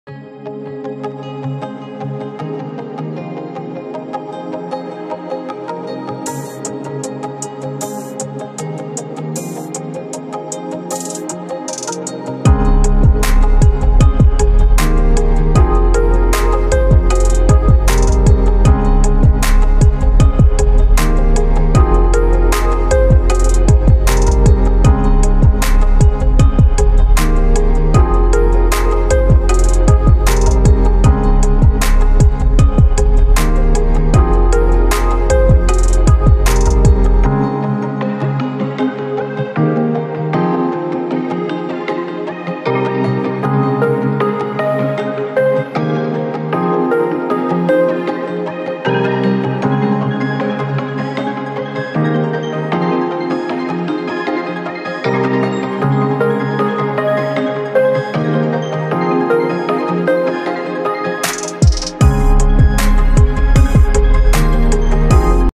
before after painting for home wall banty betul
hello friends is video me humne apko before after painting ke baare me btaya hai video accha lge to like kare thank you so much ❤️🙏💓